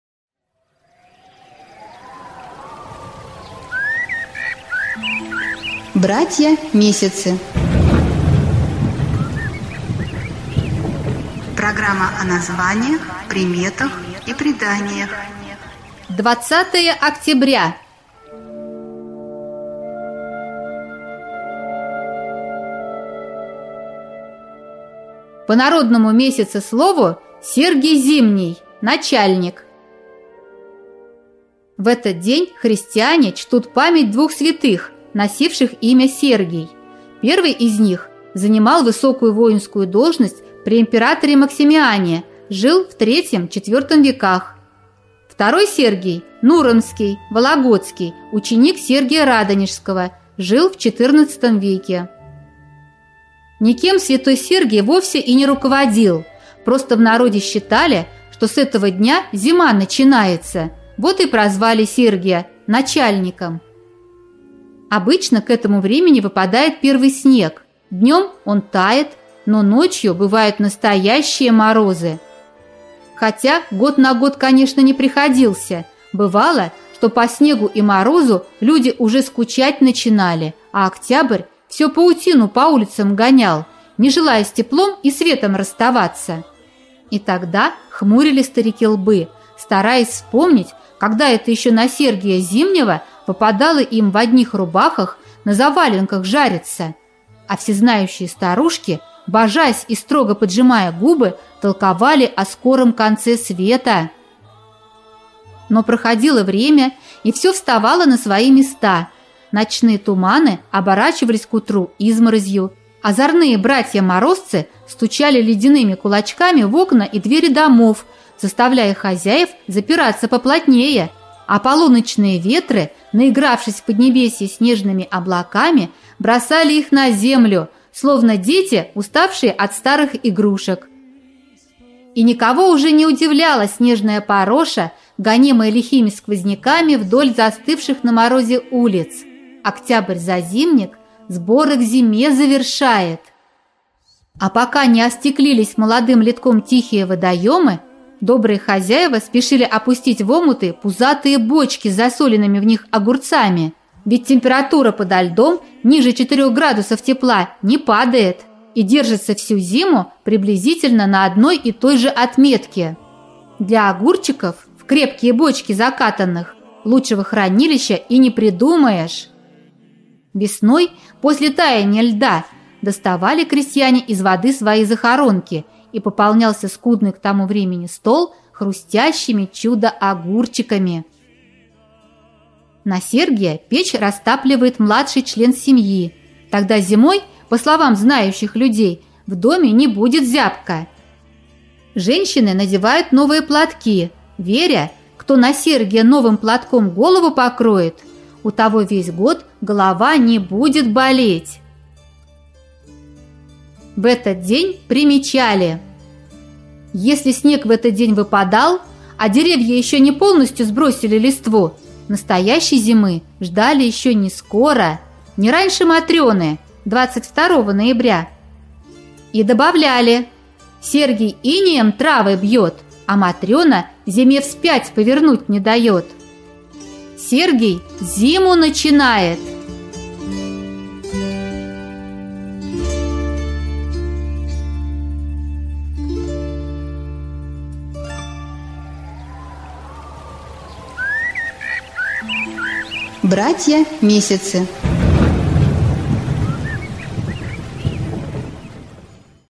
20-е октября. Братья месяцы - Сергий Зимний, Начальник
Программа о названиях, приметах и преданиях.
По народному месяцеслову: Сергий Зимний, Начальник.

В этот день христиане чтут память двух святых, носивших имя Сергий. Первый из них, занимал высокую воинскую должность при императоре Максимиане, жил в 3-ем — 4-ом веках. Второй Сергий Нуромский (Вологодский), ученик Сергия Радонежского. Жил в 14-ом веке.

Никем святой Сергий вовсе и не руководил, просто в народе считали, что с этого дня зима начинается, вот и прозвали Сергия начальником. Обычно к этому времени выпадает первый снег, днем он тает, но ночью бывают настоящие морозы.

Хотя год на год, конечно, не приходился. Бывало, что по снегу и морозу люди уже скучать начинали, а октябрь все паутину по улицам гонял, не желая с теплом и светом расставаться. И тогда.

Продолжение слушайте в аудиозаписи.